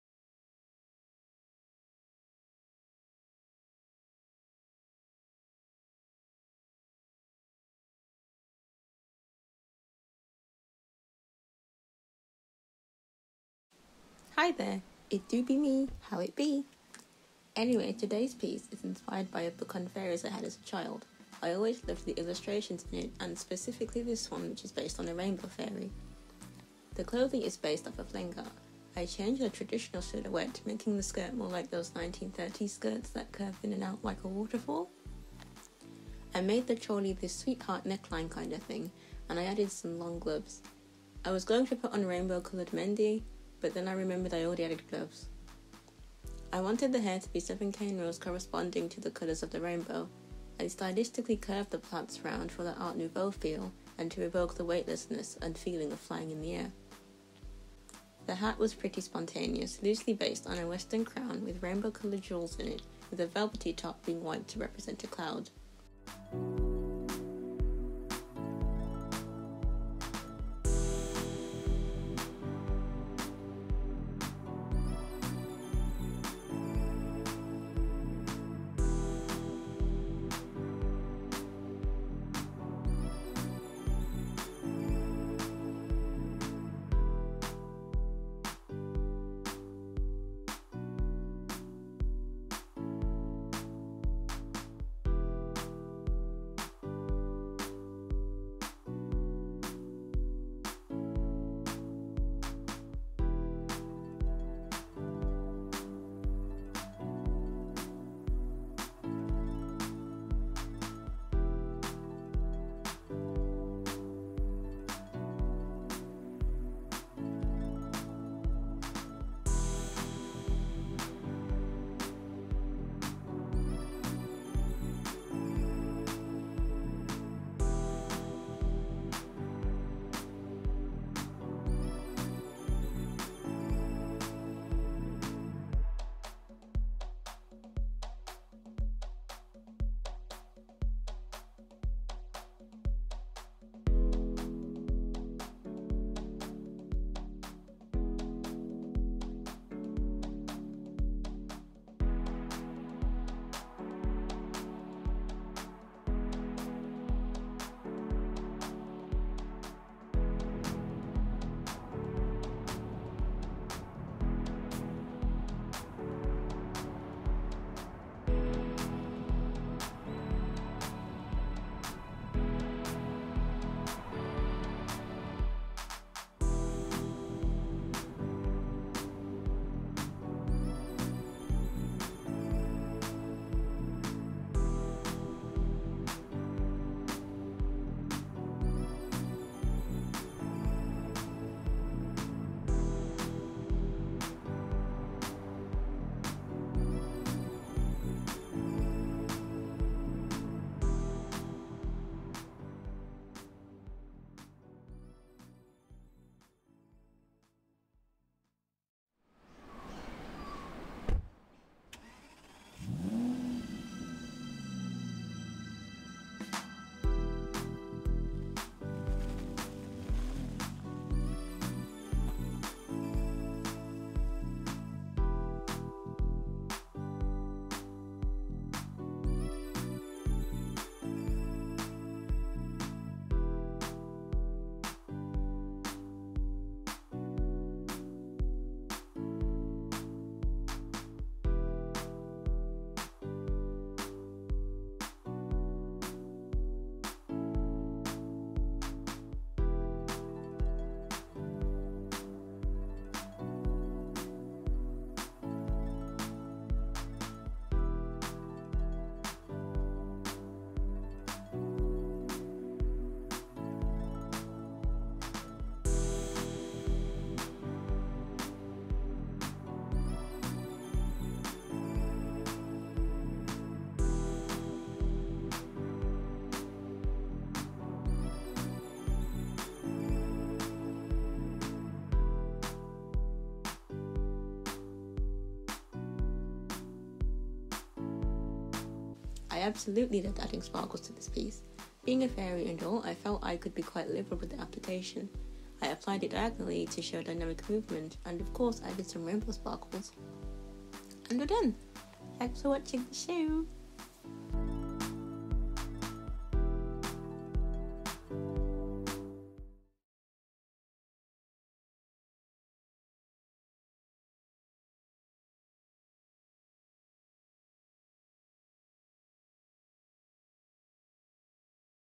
Rainbow fairy art time-lapse
I decided to make myself a rainbow fairy inspired by fairy illustrations in old books I've gotten.

Taking inspiration from traditional Indian clothing and Afro hairstyles to create a unique design.

This one was quite fun. If I get round to it I might create other fairy designs.

Social stuff